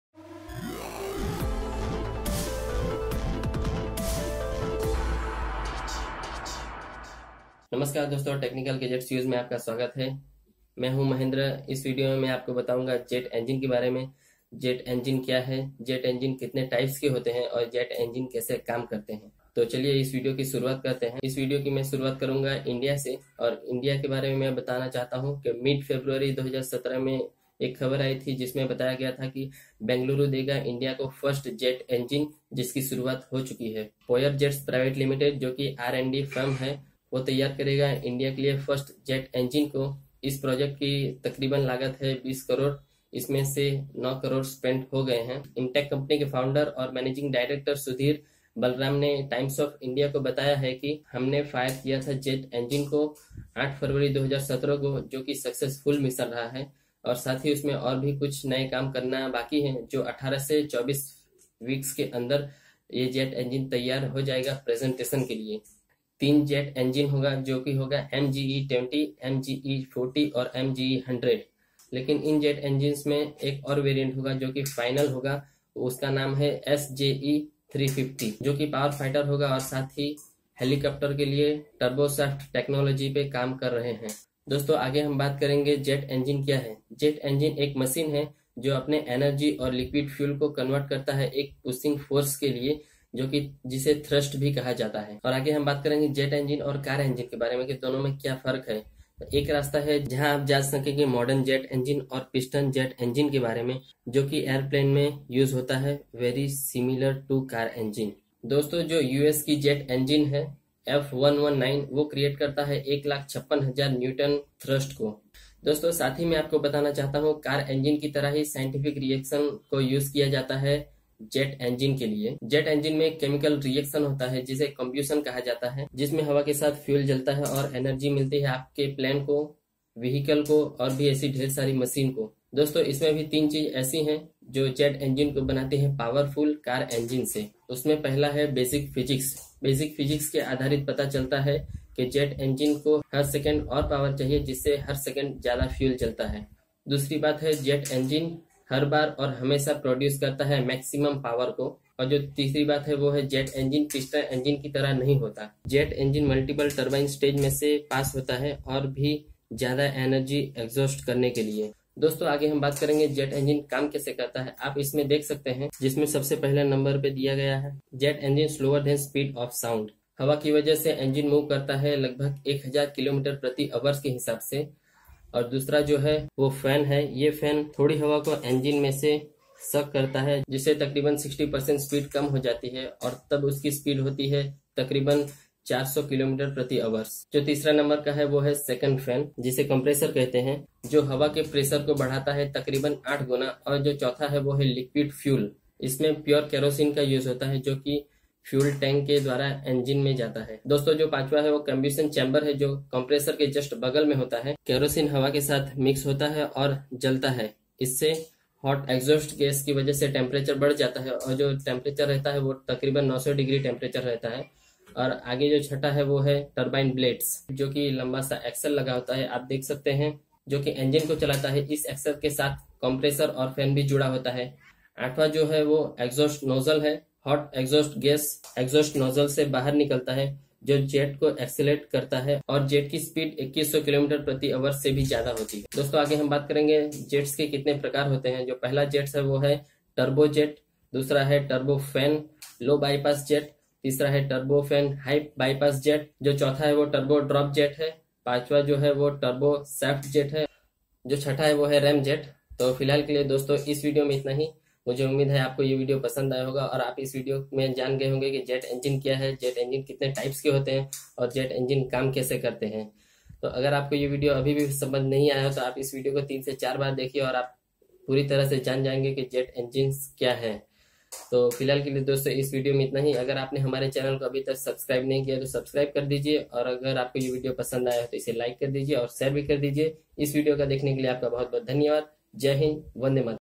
What Is Jet Engine How It Is Work And Type Of Jet Engines
Namaskar Dosto Technical Gadgets Use Me Aapka Swagat Hai.
Dosto Is Video Me Mai Apko Bataunga,
Jet Engine Kya Hai,
Jet Engine Kitane Types Ke Aate Hai Aur Jet Engine Kaam Kaise Karte Hai.
Dosto Agar Aapko Ye Video Pasand Aaaye To Like Aur Share Jaroor Kariyega,

Agar AApka Koi Sawal Ya Sujhav Ho to Comment Jaroor Kariyega.